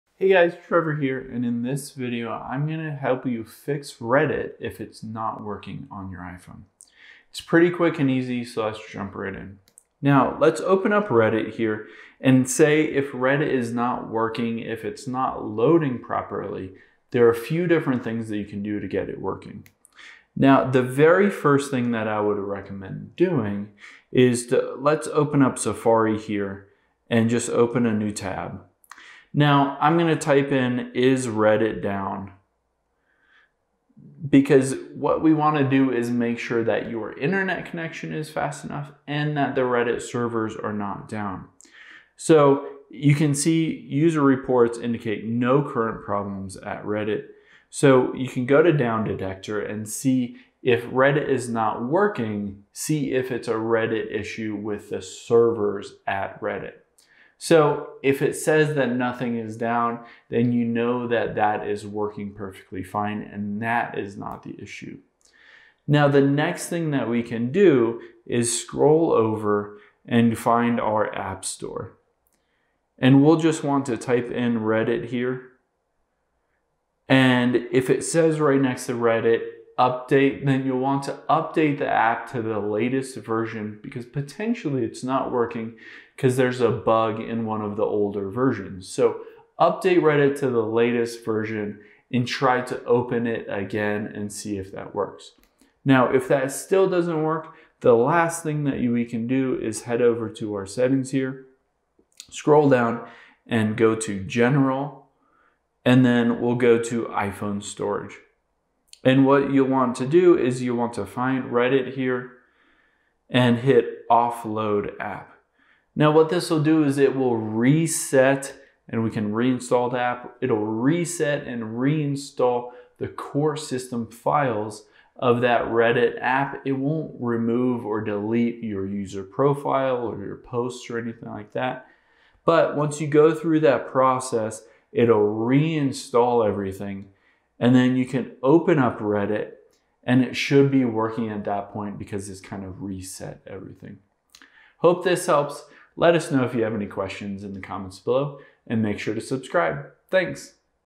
How To Fix Reddit Not Working
Let's fix the Reddit app if it's not working or not loading properly on your iPhone or Android.